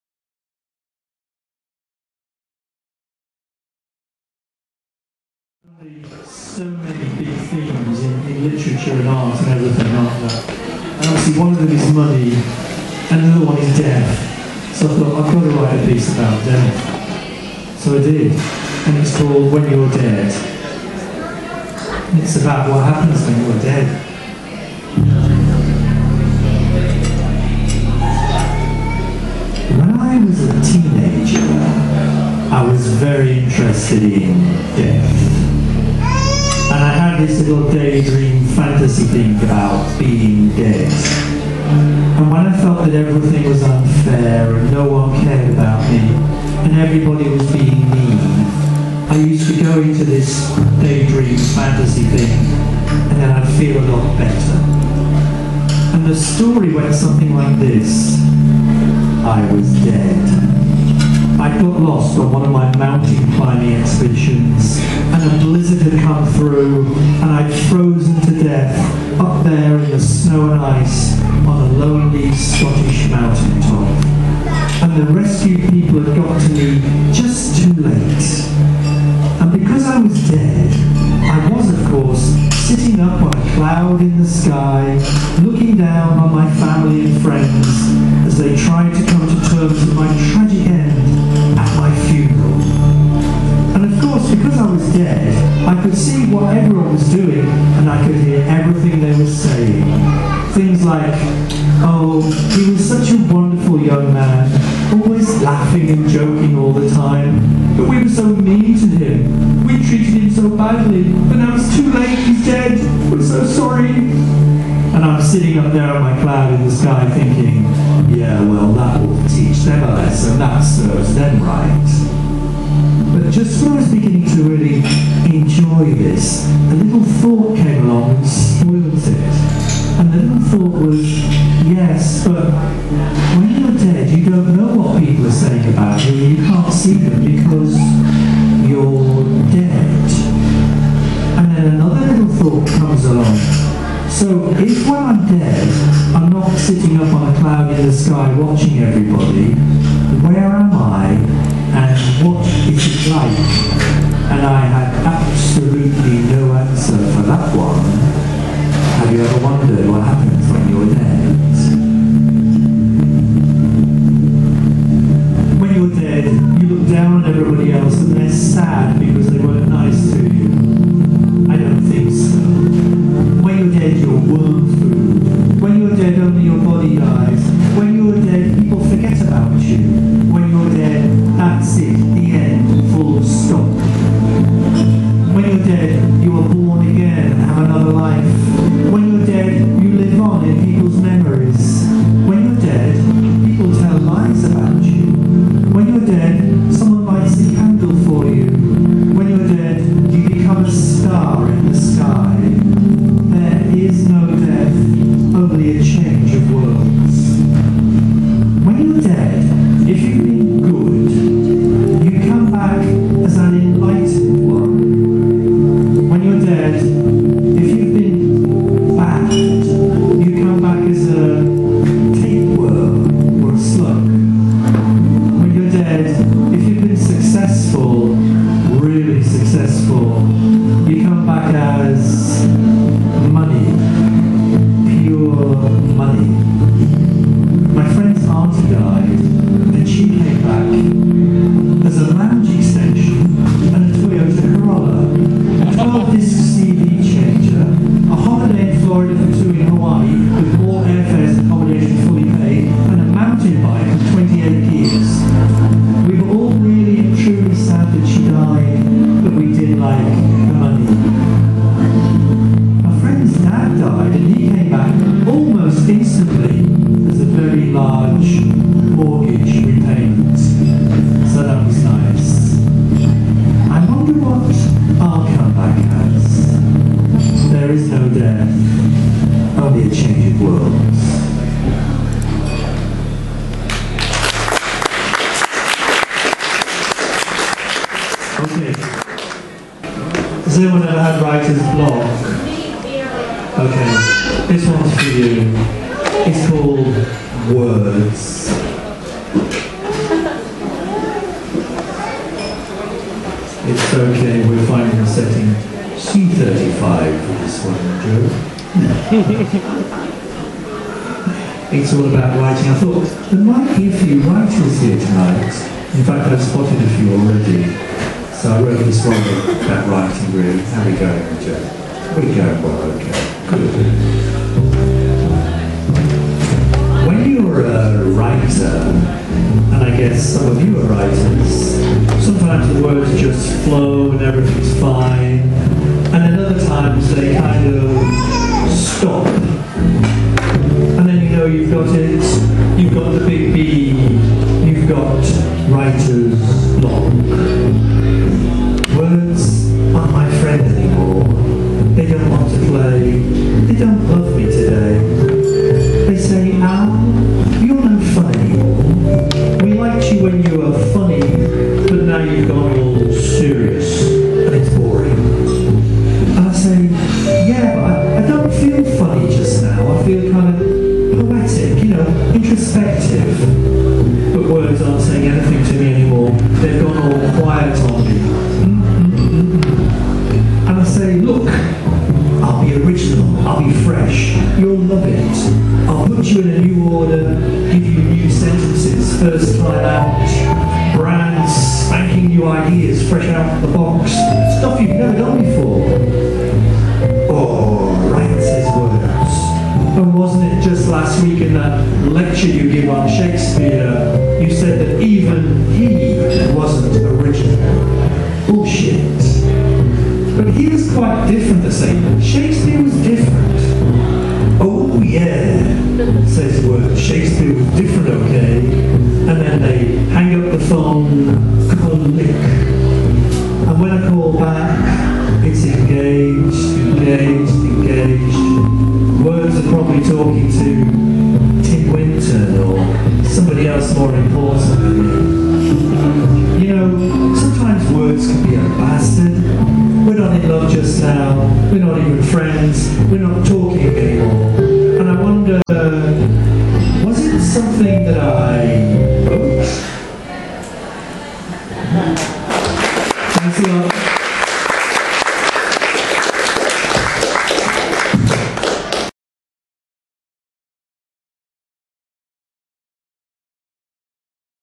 Hancock&Neave2
Alan Hancock and Joanna Neave performing live.The Second clip from Voicebox Fremantle recorded at Clancy's fish pub, Western Australia. Monday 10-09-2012